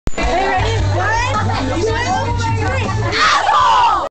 1 2 3.. ASSHOLE!
we were at a friends house one night... [drunk].. enough said LOL